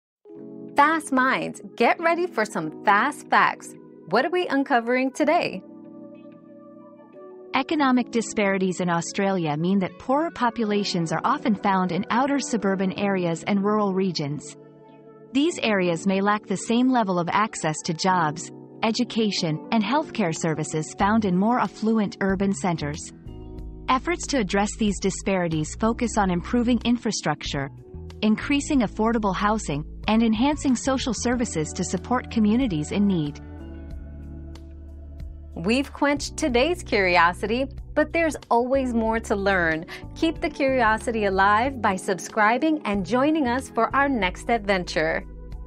Where do poor people live in Australia?
Mapping Poverty in Australia: Where Do Poor People Live? 👉 Poverty in Australia 👉 Discover the geographical distribution of poverty in Australia and the challenges faced by disadvantaged communities in accessing essential services and opportunities.

Laura S. Harris (2024, March 22.) Where do poor people live in Australia?
    🌐 AskAbout.video